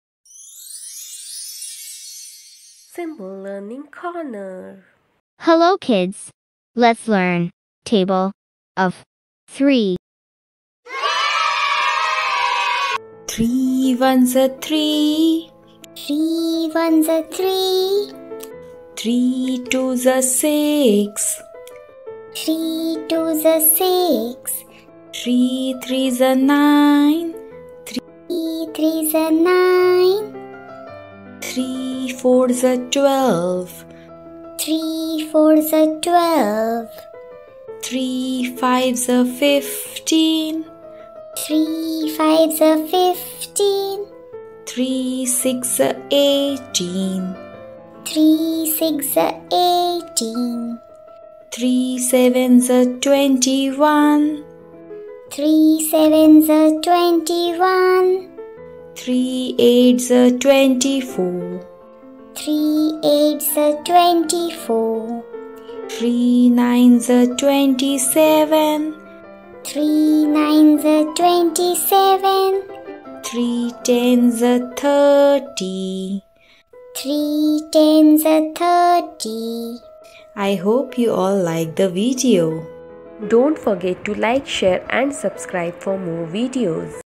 Table of 3 | 3×1=3 | Learn Multiplication Table of 3 × 1 = 3 | Rhythmic Table of 3 | Maths Tables
Table of 3 | Learn Multiplication Table of 3 × 1 = 3 | Rhythmic Table of 3 | Maths Tables

Hi kids,
let's learn Table of 3 and Multiplication  altogether..
This video is a part of vocabulary video for kids to learn about Tables, in a Rhythmic way.
The easy way to learn Multiplication Table of 3 , this is an excellent resource for preschool and primary education..